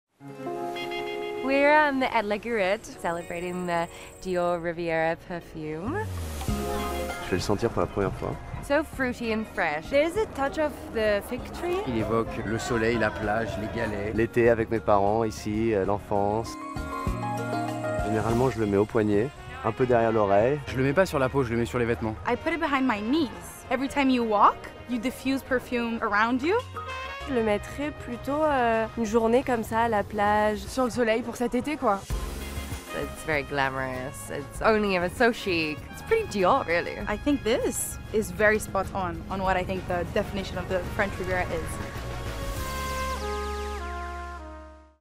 Embark on a sensory journey with Dioriviera at La Guerite
Embrace the allure of the French Riviera as we embark on a sensory journey. Join us in celebrating the launch of DiorLaCollectionPrivee fragrance: Dioriviera, an exquisite olfactory masterpiece crafted by the visionary Francis Kurkdjian, Perfume Creation Director. Alongside the presence of Carla Ginola, Georgia Fawler, Cesar Domboy, Jeremie Laheurte, and Cassandra Cano.